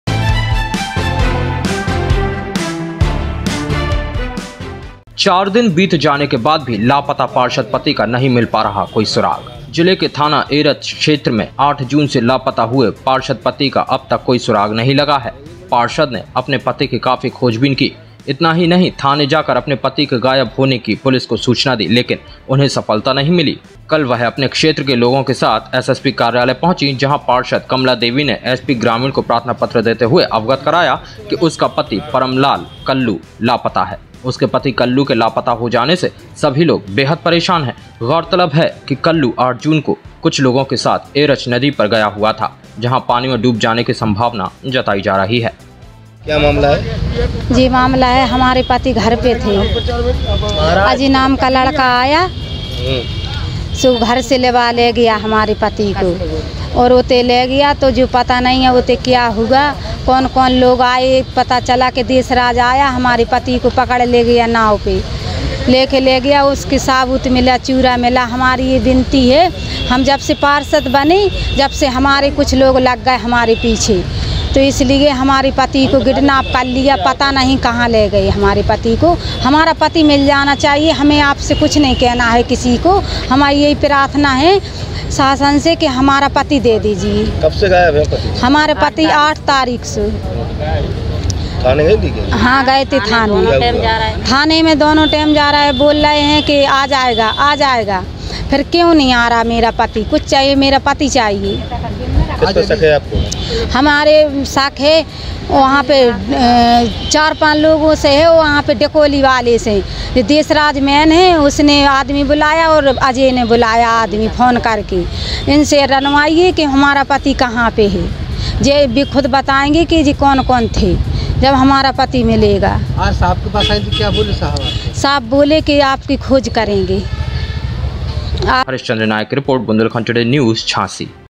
झाँसी: चार दिन बीत जाने के बाद भी लापता पार्षद पति का नहीं मिल पा रहा कोई सुराग
झाँसी: जिले के थाना एरच क्षेत्र में 8 जून से लापता हुए पार्षद पति का अब तक कोई सुराग नहीं लगा है,पार्षद ने अपने पति की काफी खोजबीन की,इतना ही नहीं थाने जाकर अपने पति के गायब होने की पुलिस को सूचना दी,लेकिन उन्हें सफलता नहीं मिली। आज वह अपने क्षेत्र के लोगों के साथ एसएसपी कार्यालय पहुंची, जहां पार्षद कमला देवी ने एसपी ग्रामीण को प्रार्थना पत्र देते हुए अवगत कराया कि उसका पति परमलाल कल्लू लापता है,उसके पति कल्लू के लापता हो जाने से सभी लोग बेहद परेशान हैं,गौरतलब है कि कल्लू 8 जून को कुछ लोगों के साथ एरच नदी पर गया हुआ था जहां पानी में डूब जाने की संभावना जताई जा रही है